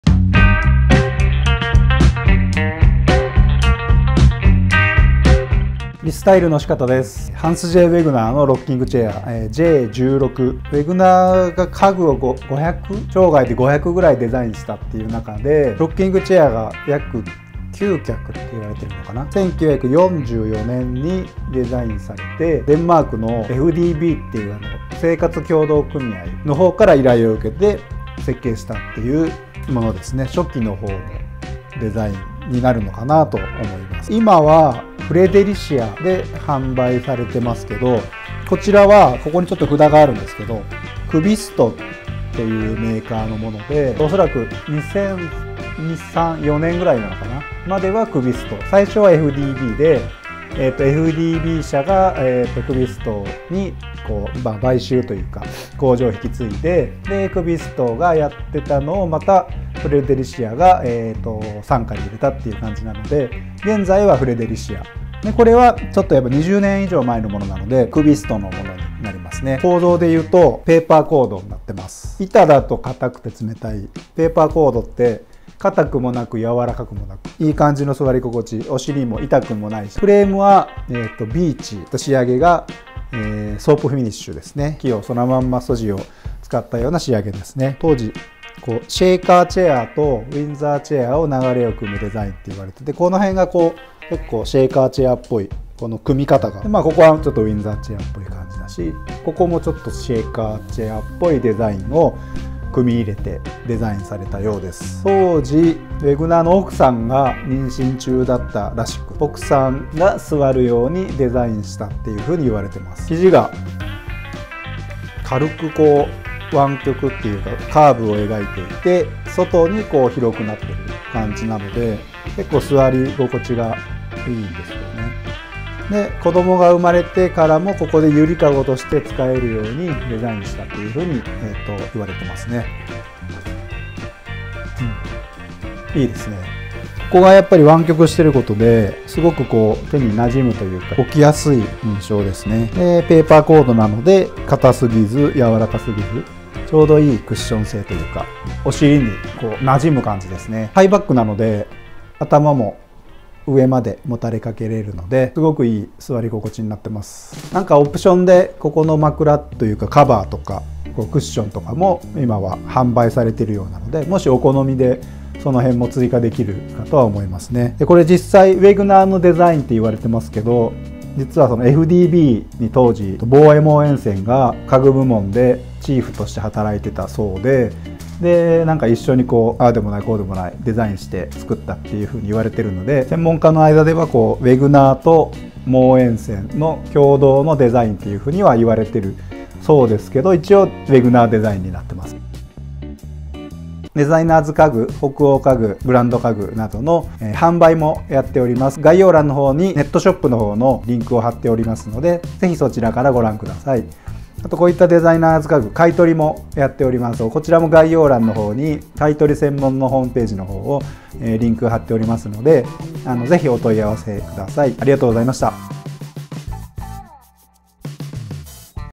ハンスJウェグナーデザインのロッキングチェア入荷！詳しく見ていきましょう。クヴィスト モブラー / Kvist Mobler J16 。
ハンス J. ウェグナーの作品の中でも大変有名な「J16」。
1944年、デンマークの生活協同組合FDBから発表されました。その後FDBはデンマークの家具メーカー クヴィストに工場を引き継ぎ、
クヴィストは現在ではフレデリシアの傘下に入っています。希少なタグまできれいに残っています。シェーカー家具の展示会に訪れていたボーエ・モーエンセンとウェグナーに、FDBの社長が直接ロッキングチェアの製作を依頼したことや、 工場で2人がプロトタイプを製作していたというFDBの職人による証言などから、ボーエ・モーエンセンとの共作ともいわれています。
大変美しいロッキングチェアです。買取も販売もしていますので興味のある方は是非リンクからご覧ください。